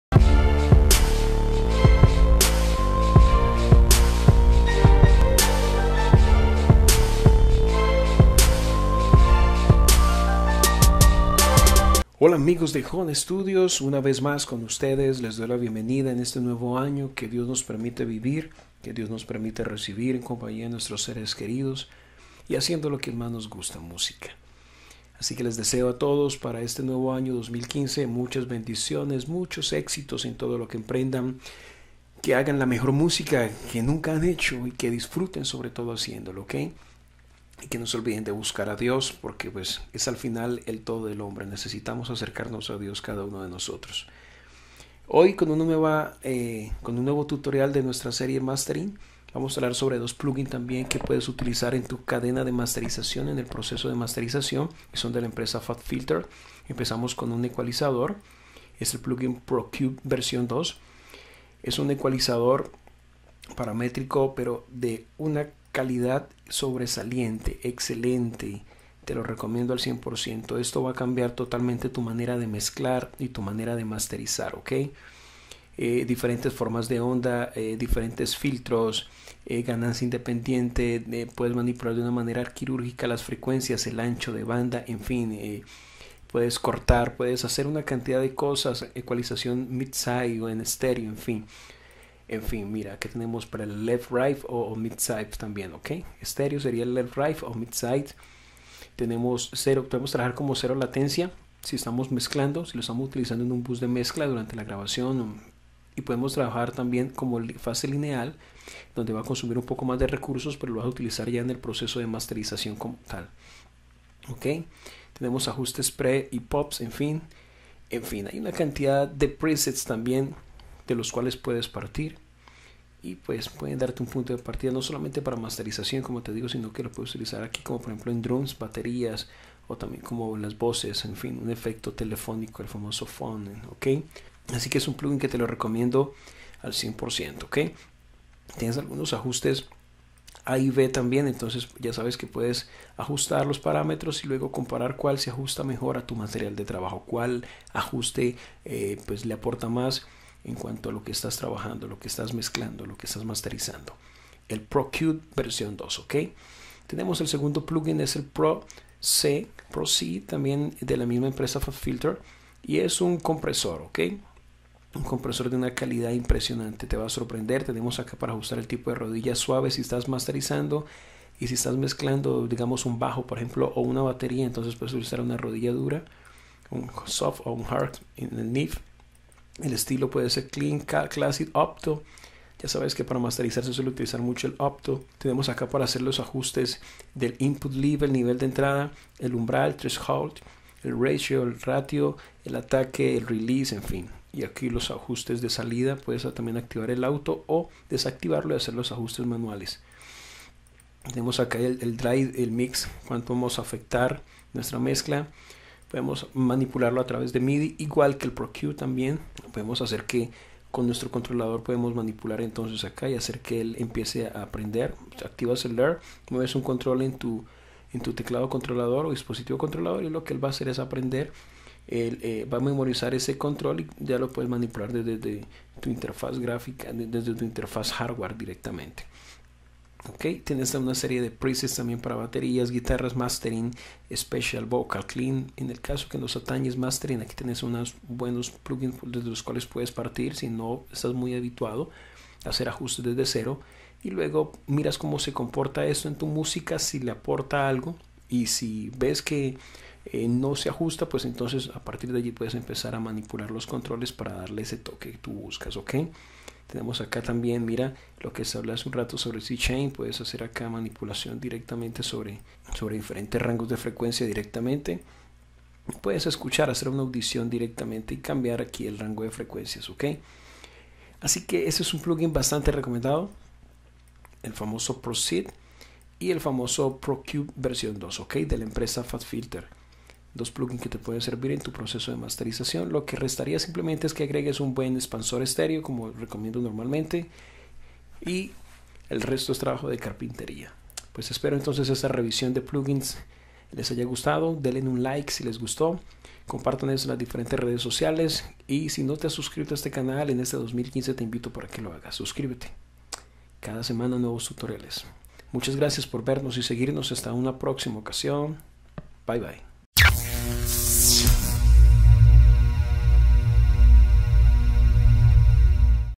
Mastering Series: Fabfilter Bundle
Fabfilter es una empresa líder e innovadora en la producción y desarrollo de plugins para producción musical y nos presenta este par de plugins que podemos utilizar en nuestros procesos de masterización musical.